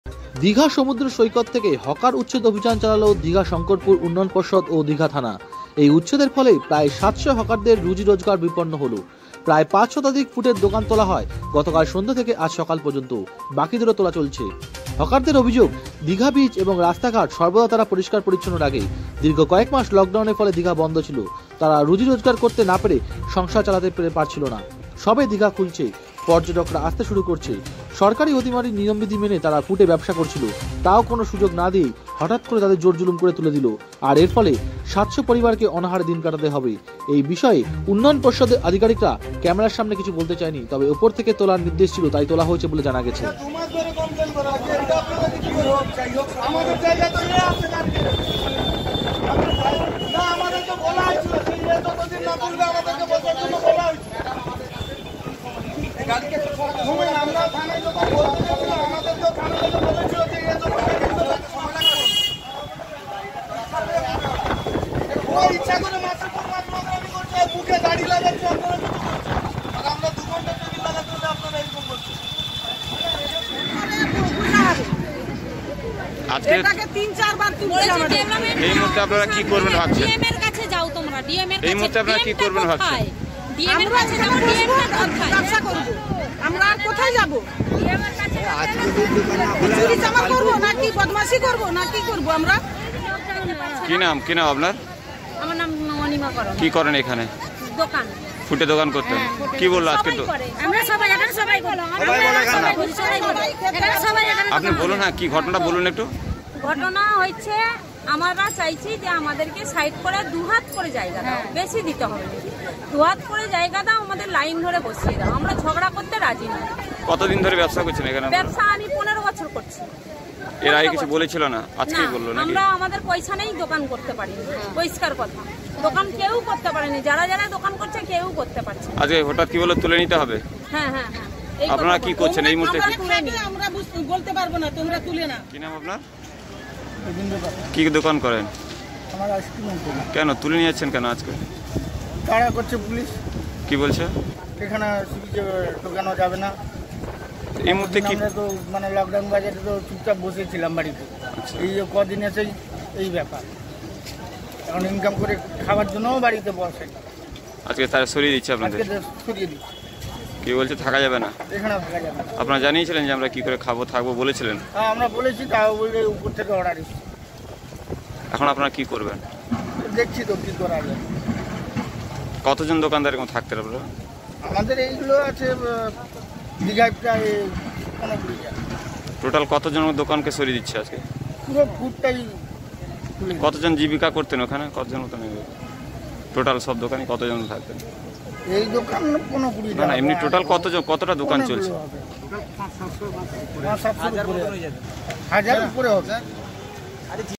সমুদ্র সৈকত থেকে হকার উচ্ছেদ অভিযান চালালো দীঘা প্রশাসন#Digha#Hocker#Seaside#
সমুদ্র সৈকত থেকে হকার উচ্ছেদ অভিযান চালালো দীঘা প্রশাসন